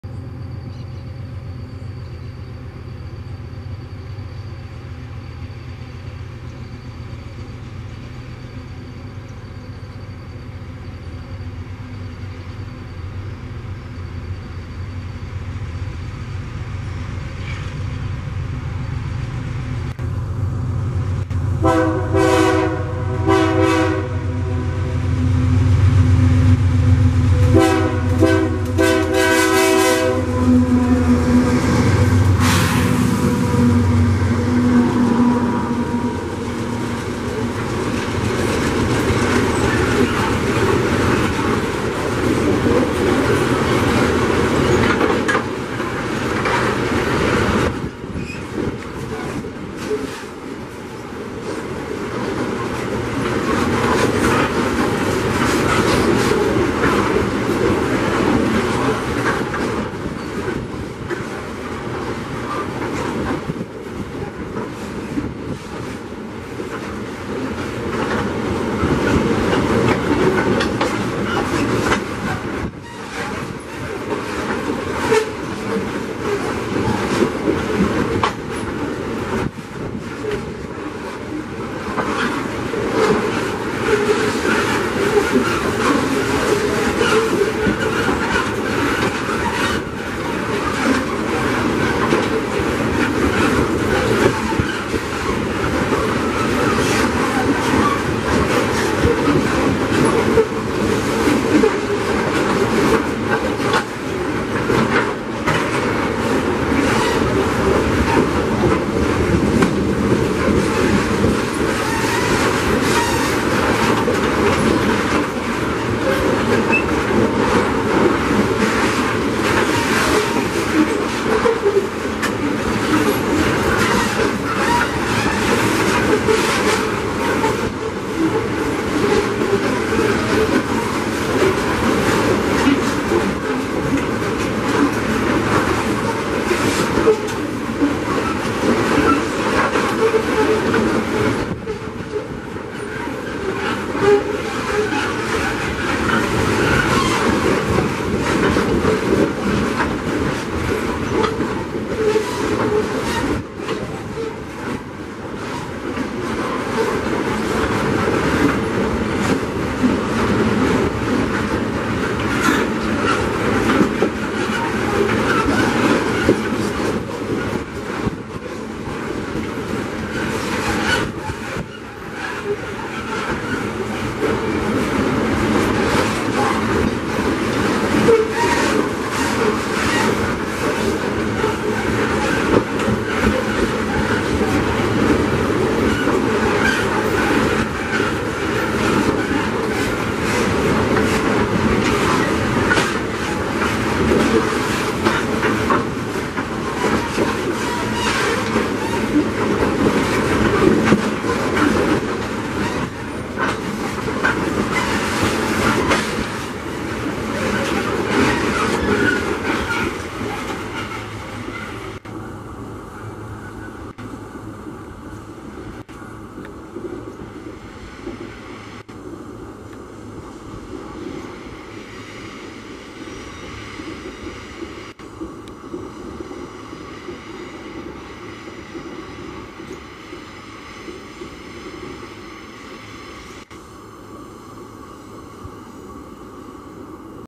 Trem NEG nas proximidades de Brumadinho
Trem prefixo NEG oriundo do patio de Sarzedo Velho na cidade de Sarzedo-Mg, liderado pelas locomotivas modelo GE AC44i 7316-7+ 3465-0 e 136 vagões GDT carregados, passando pela antiga PN do Inhotim e se aproximando do pátio de Brumadinho.